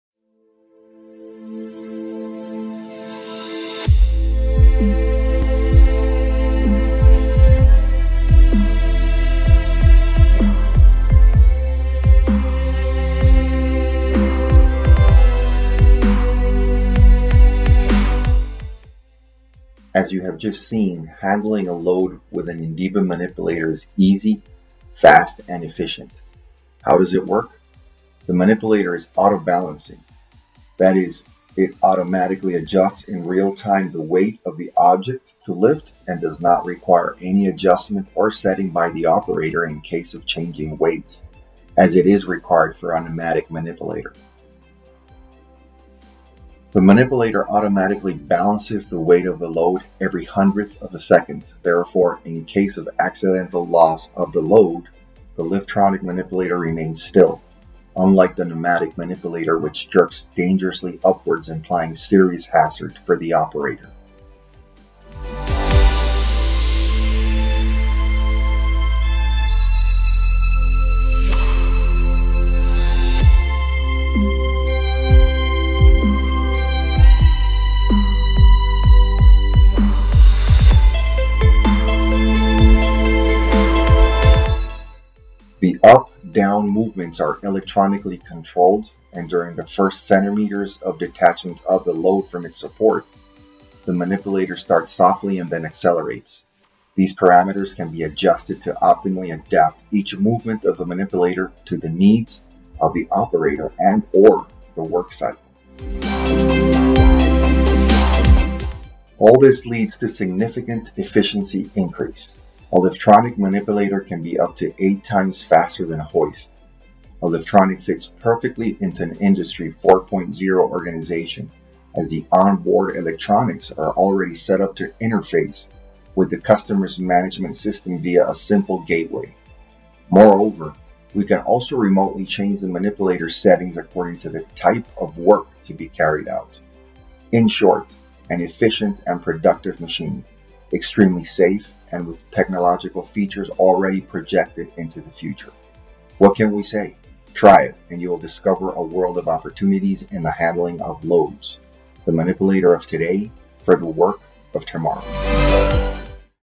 Intelligent Lift Assist Devices: how they boost your productivity and ergonomics
Extremely safe: the manipulator automatically balances the weight of the load every hundredth of a second, therefore in case of accidental loss of the load, the Liftronic manipulator remains still, unlike the pneumatic manipulator which jerks dangerously upwards implying serious hazard for the operator.

An extension of Human arm: the UP/DOWN movements are electronically controlled and during the first centimeters of detachment of the load from its support, the manipulator starts softly and then accelerates; these parameters can be adjusted to optimally adapt each movement of the manipulator to the needs of the operator and/or the work cycle. All this leads to significant efficiency increase.

Faster than a hoist: a Liftronic manipulator can be up to eight times faster than a hoist.

Industry 4.0: A Liftronic fits perfectly into an Industry 4.0 organization, as the on-board electronics are already set up to interface with the customer's management system via a simple gateway.